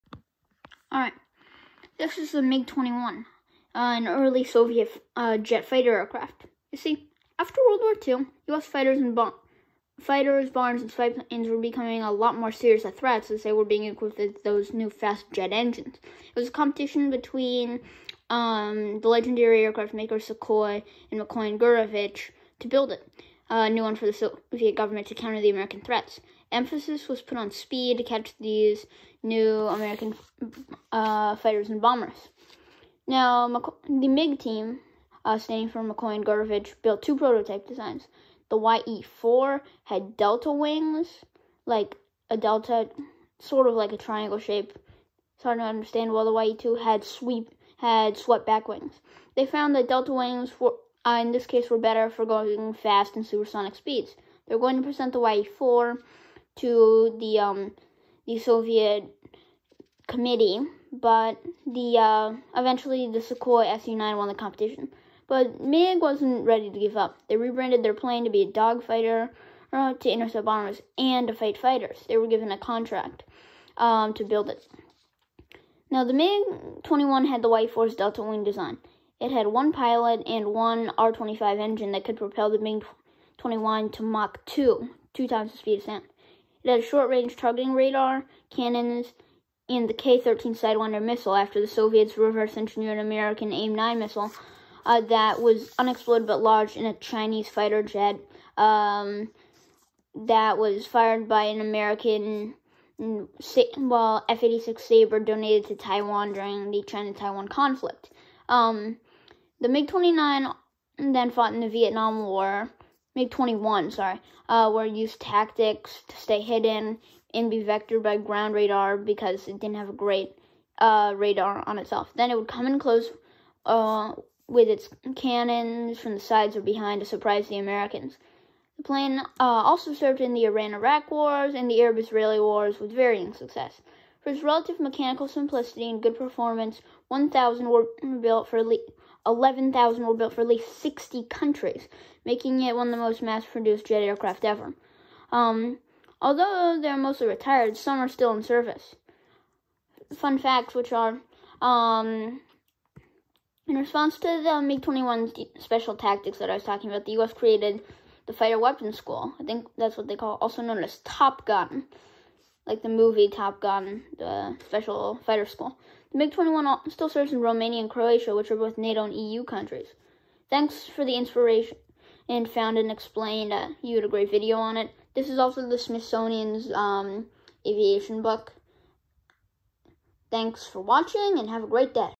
The MiG-21 “fish bed”
The results of my research about the MiG-21 fighter jet! Note: some of my sources differ from the Smithsonian book in the video about statistics. Thanks to found and explained for the inspiration. Thanks to the Smithsonian book for the pictures and heritage flight for the thumbnail.